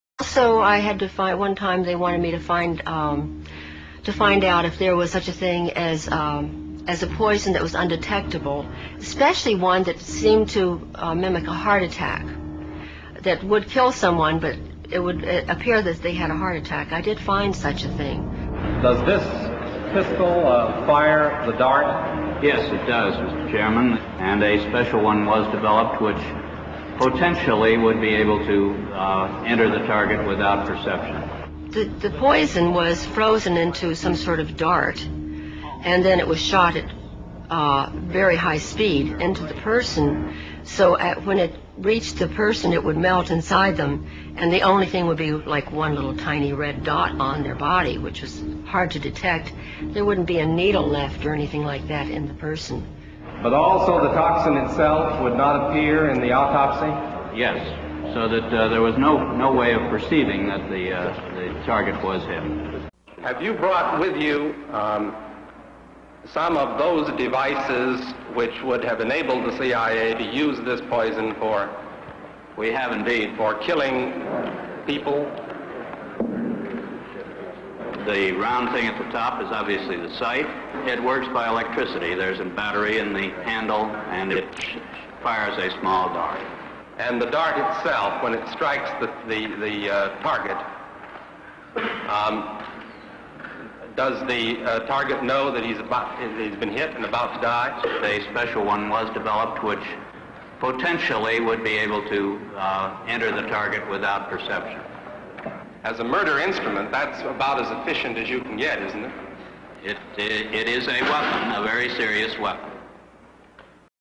Le pistolet à crise cardiaque de la CIA 1975
La CIA a révélé un pistolet «crise cardiaque» en 1975. Un pistolet à piles qui a tiré une fléchette d'eau gelée et de toxine de crustacés. Une fois à l'intérieur du corps, il fondrait en laissant seulement une petite marque rouge sur la victime où il est entré. La cause officielle du décès serait toujours une crise cardiaque.
Conspiration: 8 théories farfelues qui se sont avérées vraies

PISTOLET CIA HEART ATTAQUE

Mary Embree, qui a commencé sa carrière à la CIA en tant que secrétaire de la division Audio-Surveillance avant d'être promue au département des services techniques, dit qu'on lui a demandé de rechercher un poison qui provoquerait une crise cardiaque chez sa victime mais serait indétectable dans une autopsie. Les recherches d'Embree ont conduit au développement d'une arme top-secrète connue sous le nom de «pistolet de crise cardiaque».

Il impliquait la congélation de la toxine des mollusques mélangée à de l'eau pour former une fléchette congelée qui serait ensuite tirée du pistolet de crise cardiaque. Une fois à l'intérieur du corps, le poison se dissoudrait alors dans la circulation sanguine de la personne et provoquerait une crise cardiaque.

En 1975, le directeur de la CIA, William Colby, a présenté l'arme lors d'une audience du comité de l'Église